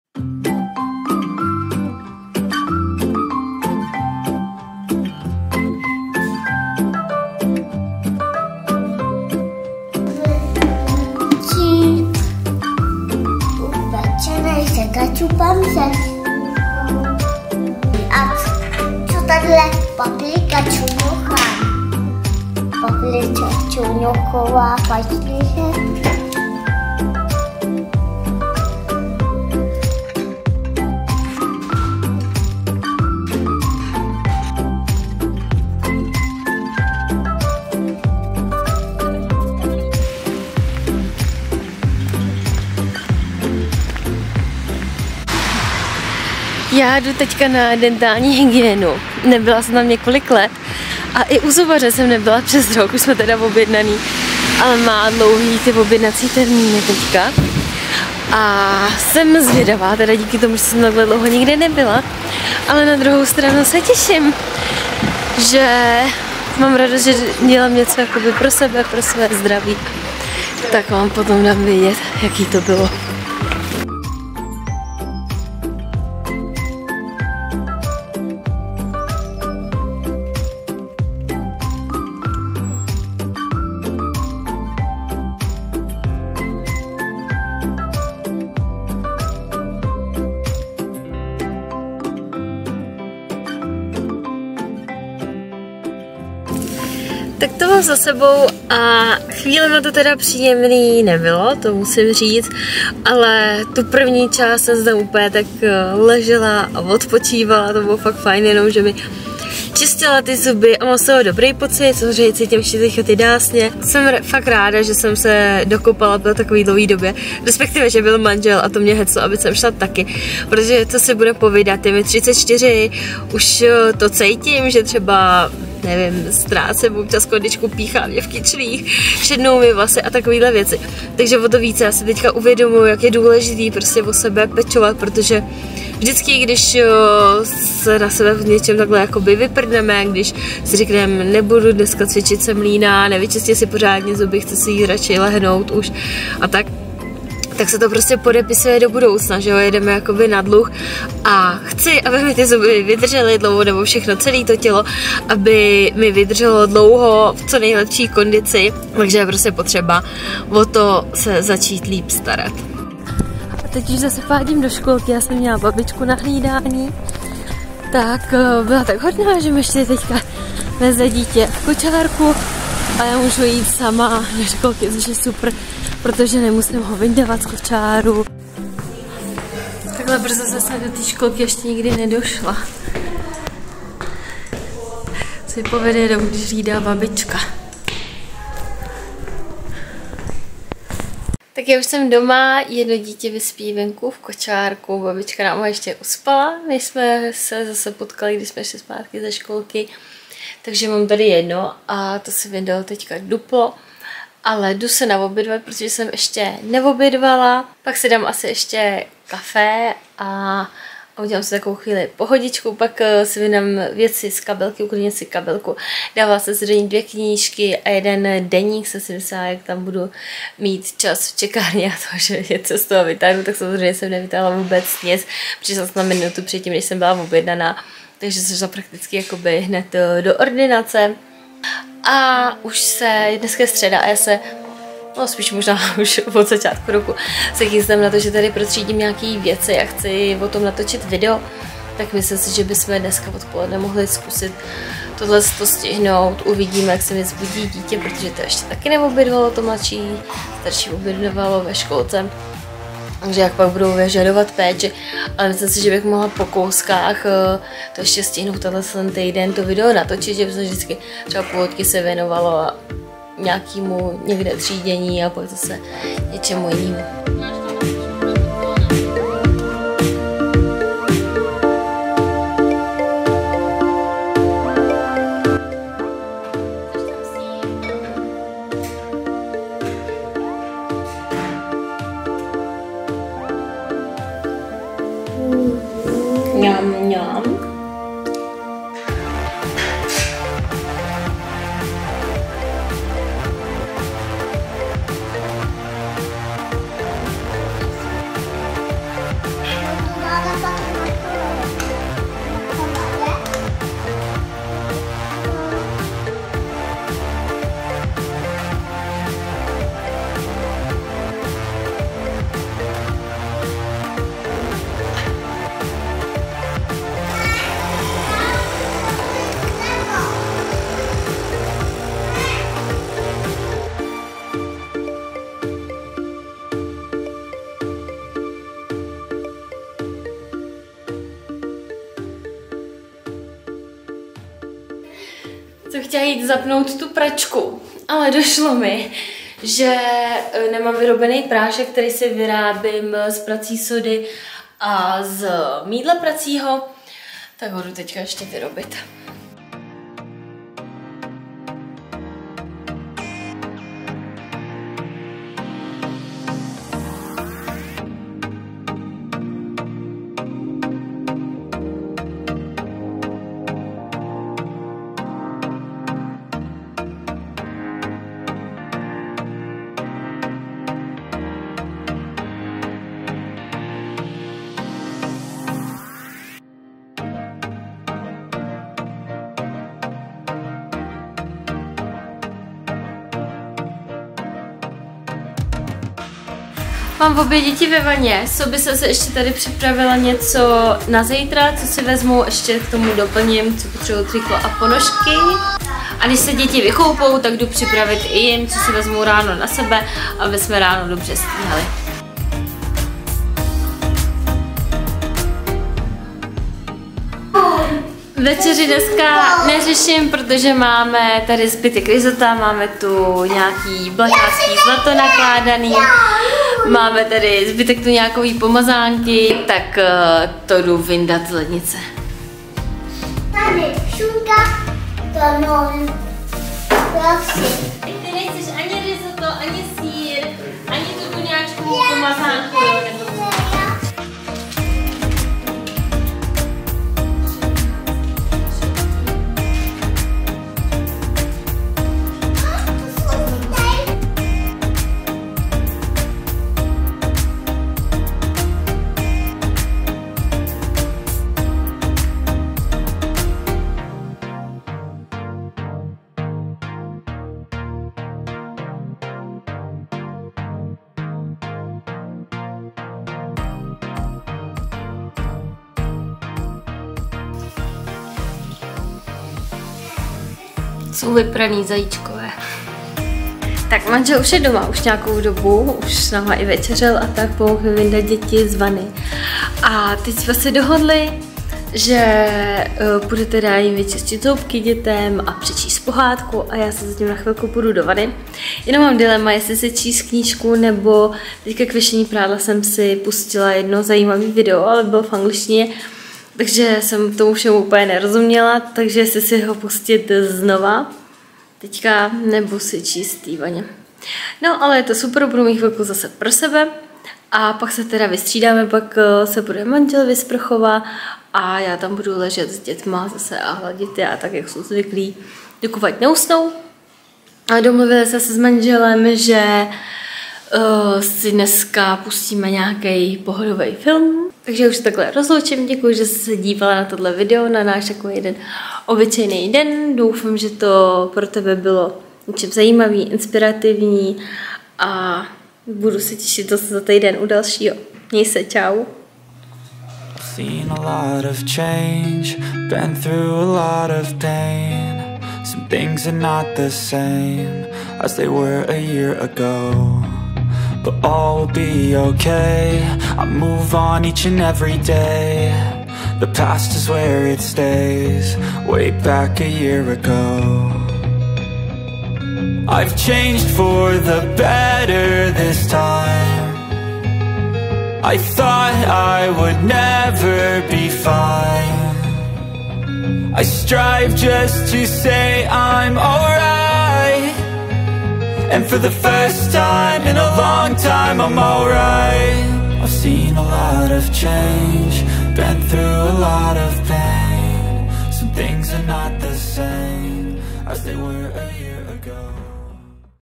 Vlog | můj všední den | dentální hygiena | jídlo, nádobí a prádlo stále dokola
První letošní vlog! Jeden všední den ze života na mateřské. Tedy až na návštěvu dentální hygieny, ta pro mě byla dost výjimečná. Naopak nekonečný kolotoč je kolem jídla, prádla a nádobí. A tak doufám, že Tě tenhle vlog nebude nudit svou všedností, ale naopak Tě potěší nahlédnutí do mého obyčejného života :)